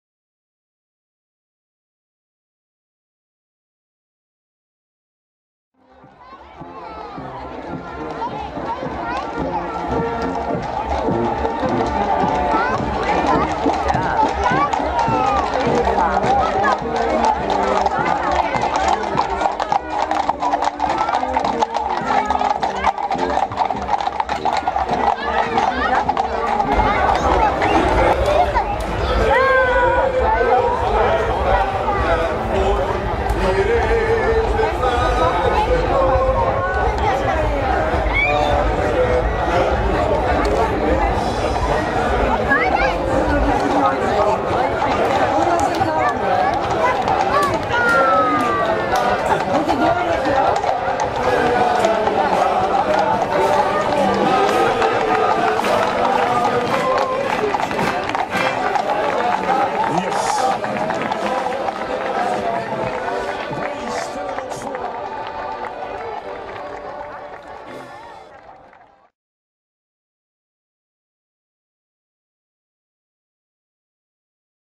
Bokbieroptocht 2016 - Fluisterbootschippers
Op zondag 9 oktober 2016 was er weer de Bokbierdagoptocht in Zutphen.
De dames van de schippers zaten op de koets en de fluisterbootschippers zaten traditiegetrouw weer in de boot.
Deze video is een onderdeel van de reportage gemaakt door Fot- Video- Zutphen.